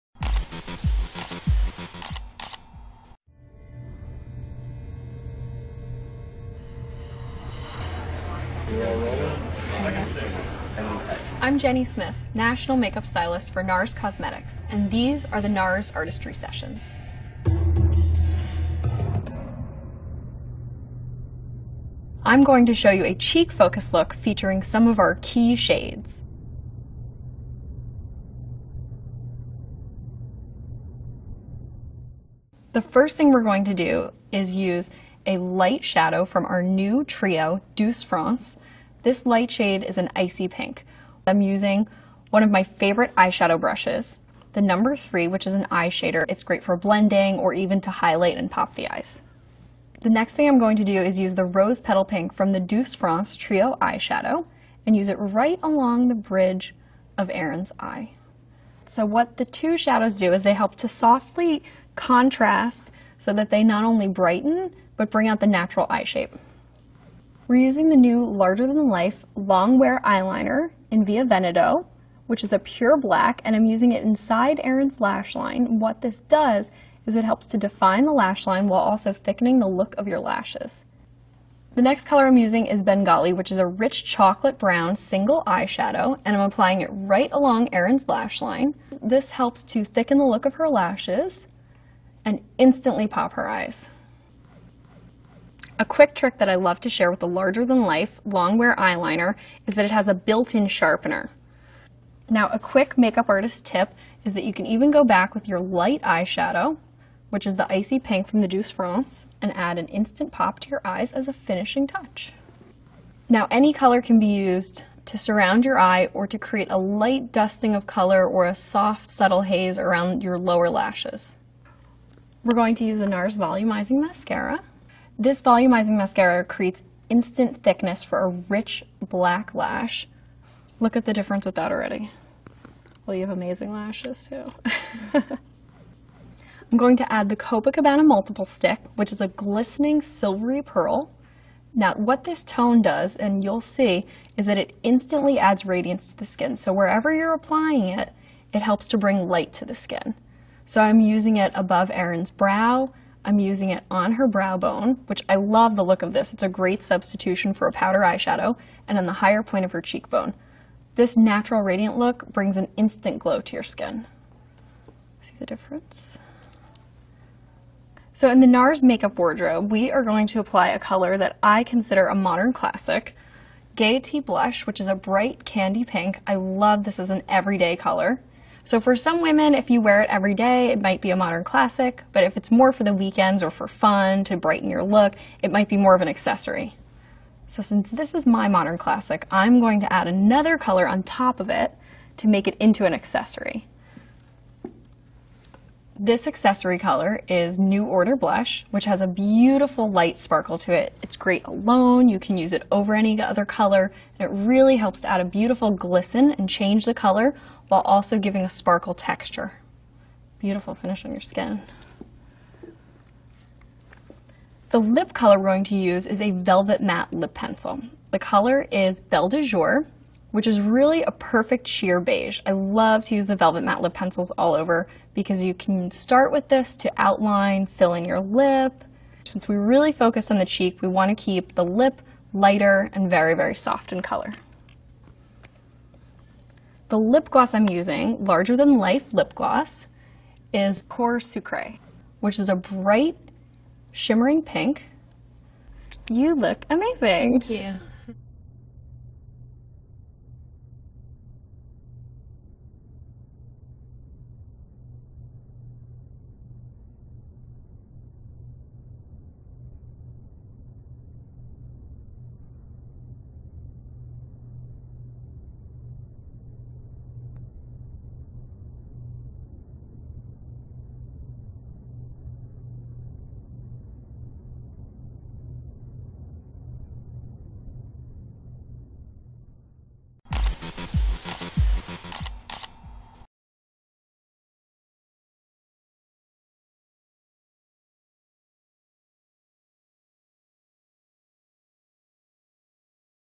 NARS | Artistry Sessions - Cheek Focus
NARS Make-Up stylist Jenny shows us how to create a subtle make-up look focusing on adding emphasis to the cheeks!